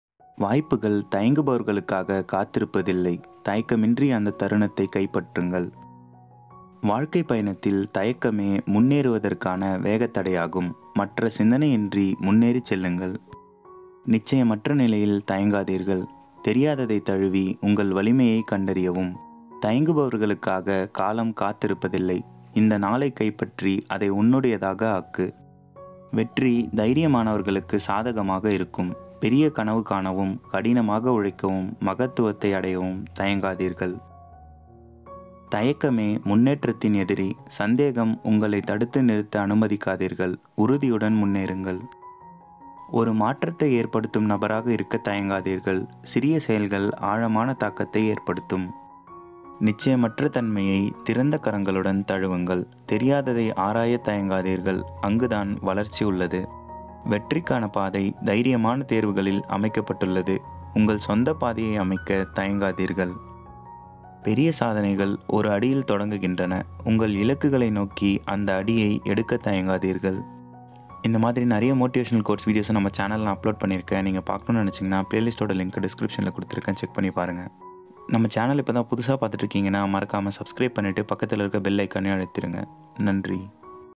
எதற்கும் தயங்காதே💥 | Feel Positive Tamil
எதற்கும் தயங்காதே
Tamil Motivational Quotes
Powerful Motivational Quotes to Overcome Hesitation and Achieve Success